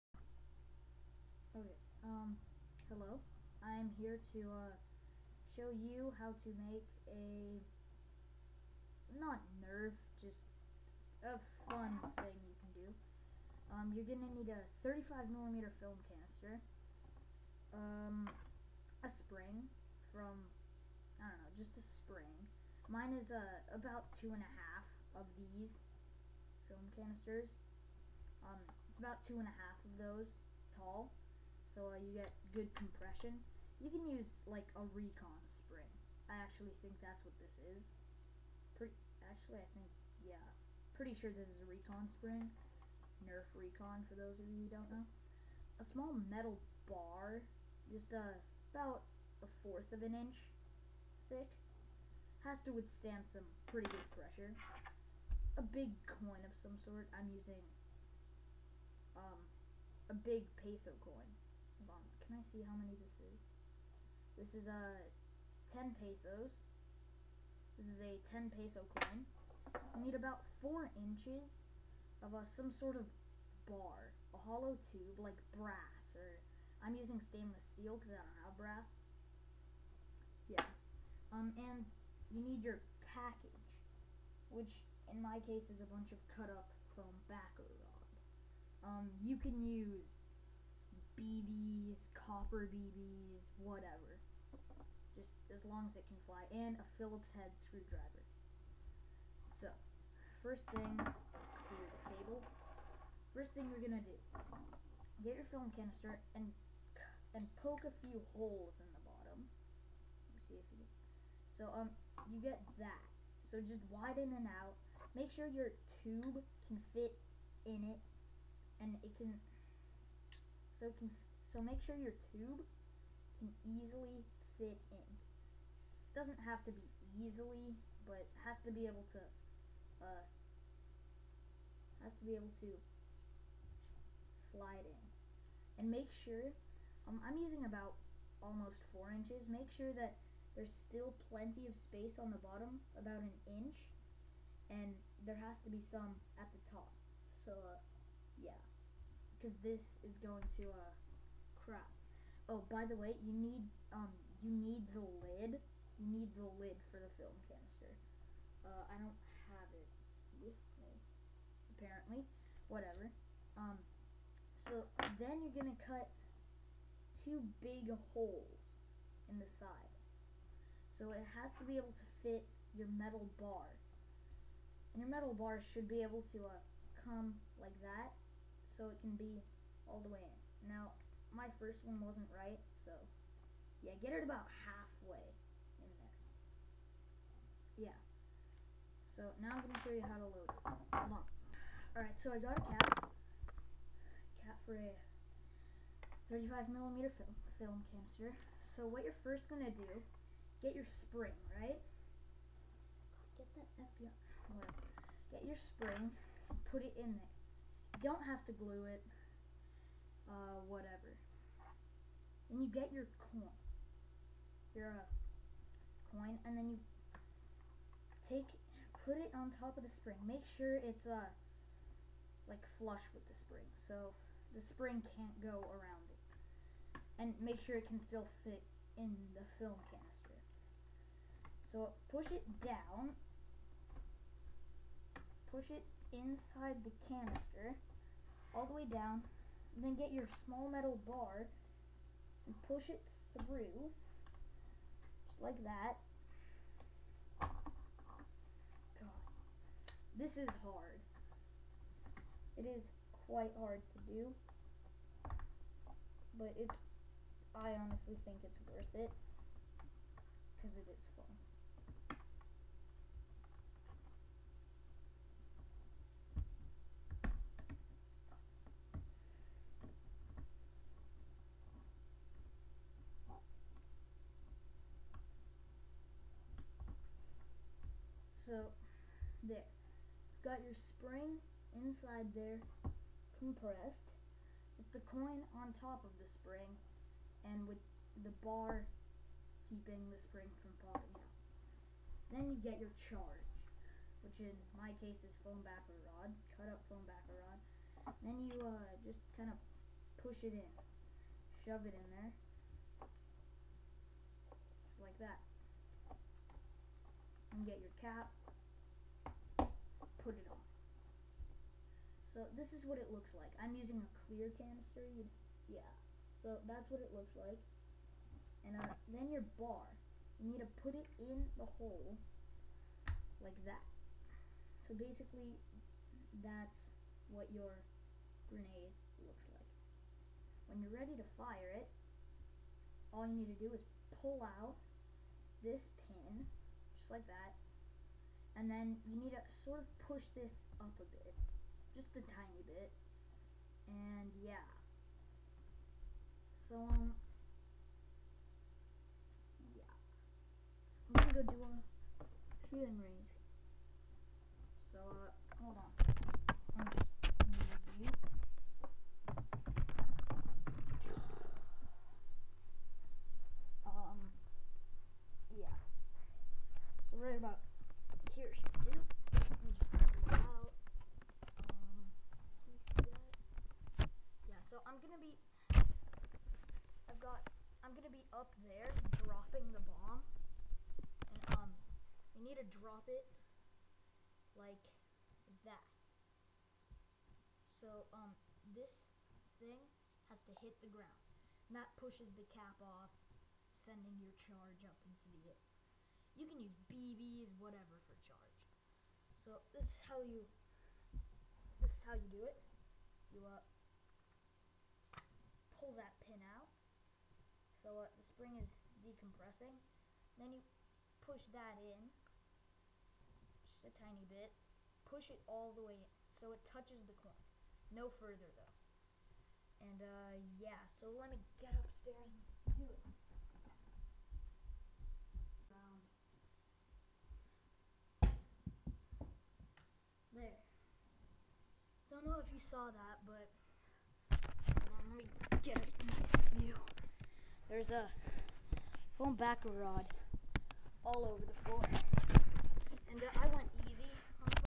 How To Make A NERF Film Canister Grenade
this can be used with really any projectile...
like bbs, cut up nerf darts, anything that fits in the canister really.
i also might make a launcher.